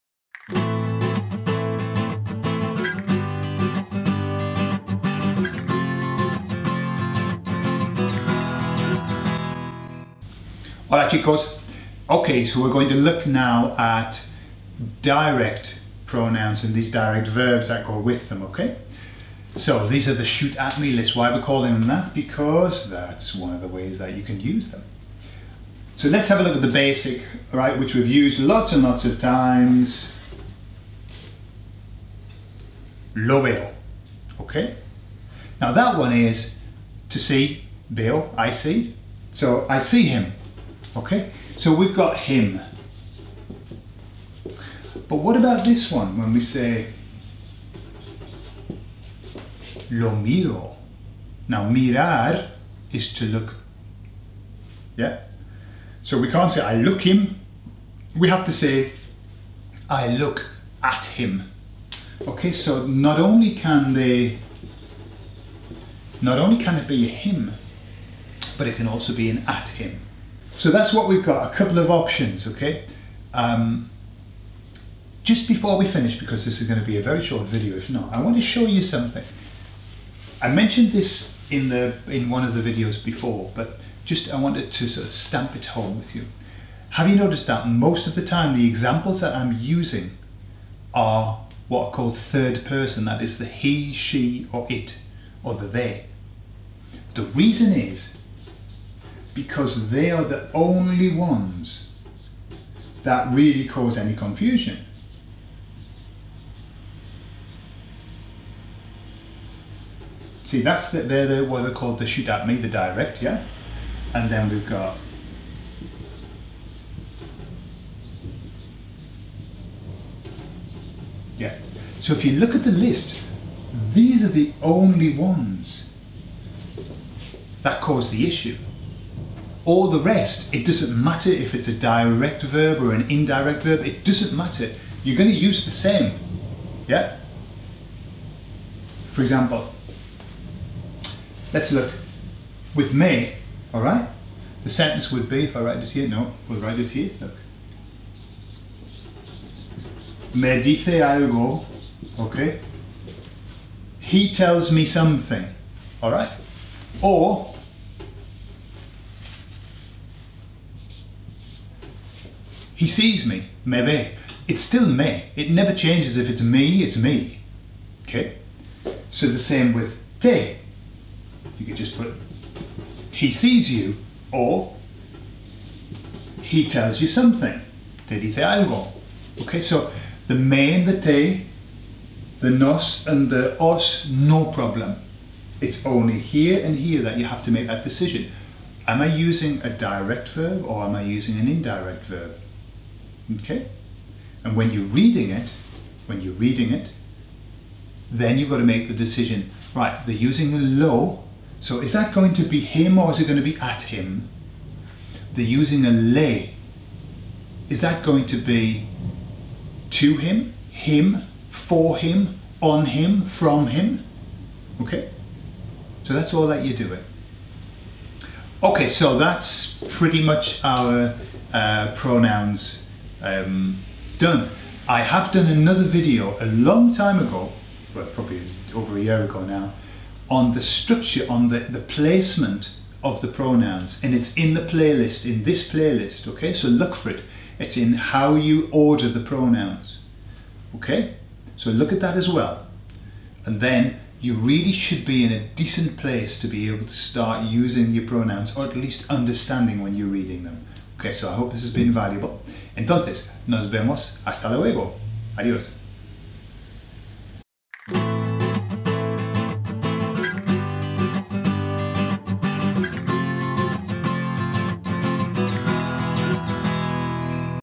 The Uses of Spanish DIRECT Pronouns LightSpeed Spanish
we are pleased to offer you a comprehensive lesson on the way that Spanish direct pronouns relate to what we say in English. Special tip! The ones to watch out for.